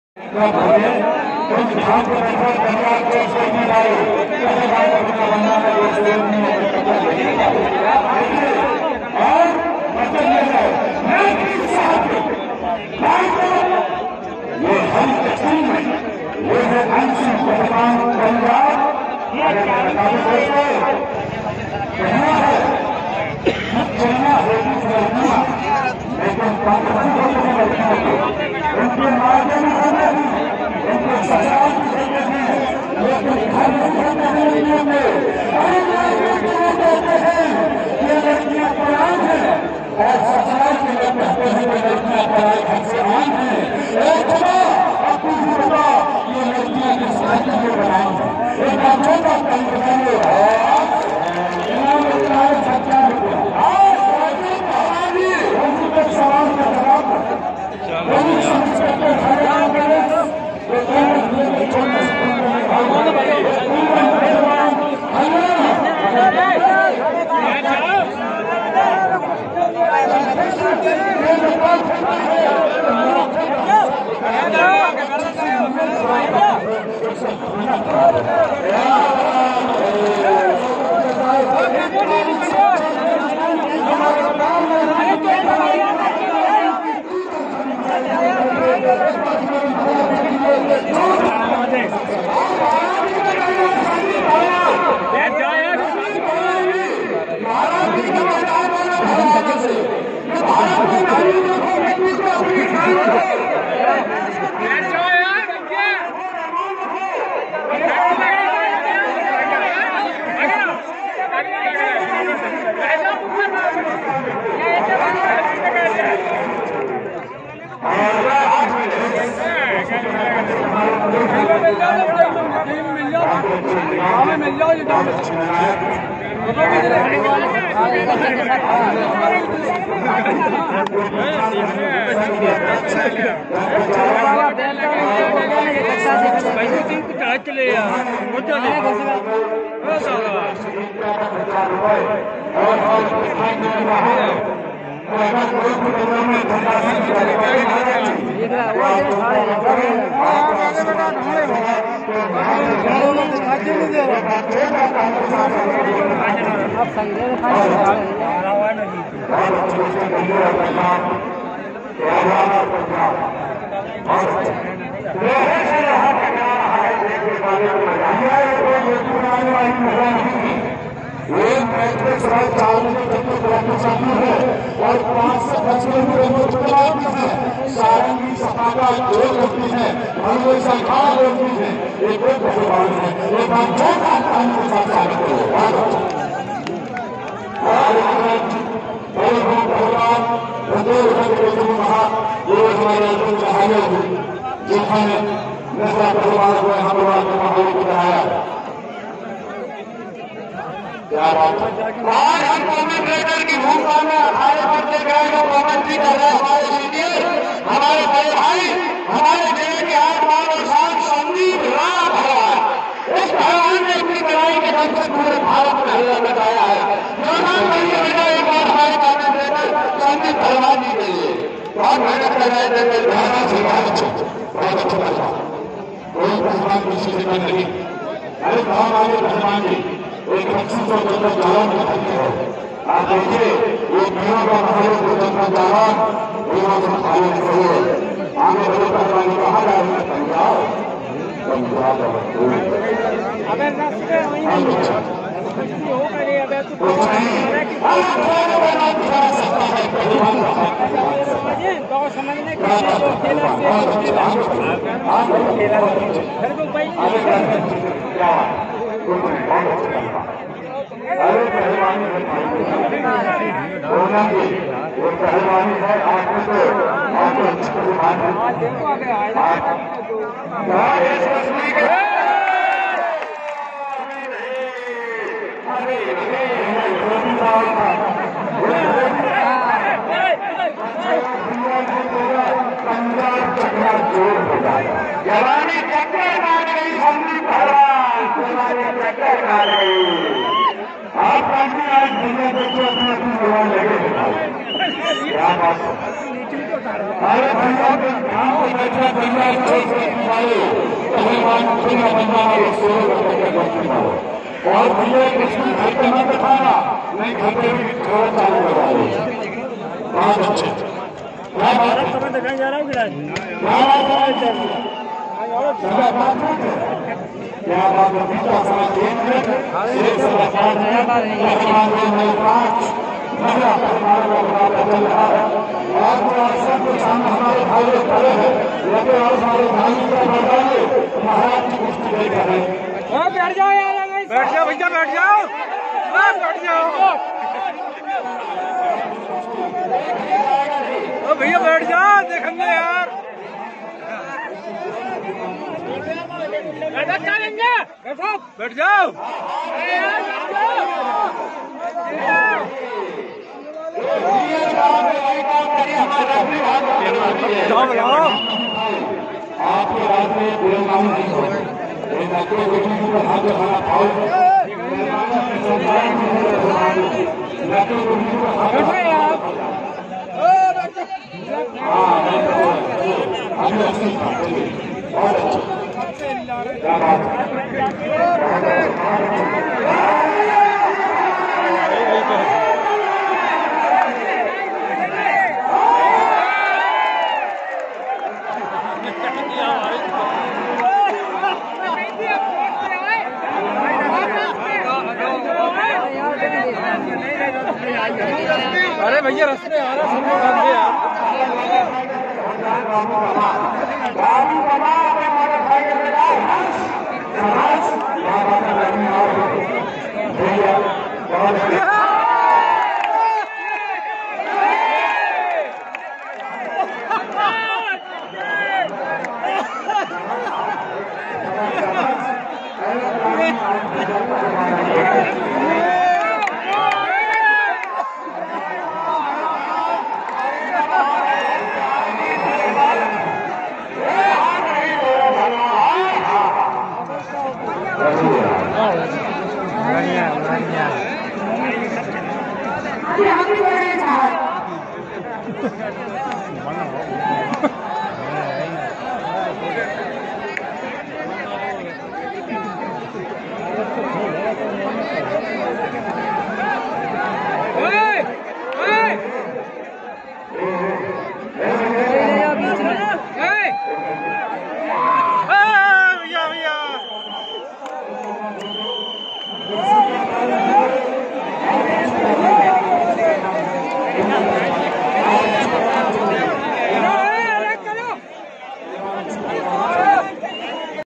हरियाणा की महिला S.i मीना पहलवान,ने पंजाब की सीमा पहलवान को पछाड़ा, दंगल में/live vedio,🔴
अंतर्राष्ट्रीय दंगल उत्तर प्रदेश में बाहर से आई महिला पंजाब केसरी और हरियाणा की मीणा एसआई दोनों में बहुत ही महत्वपूर्ण कुश्ती हुई,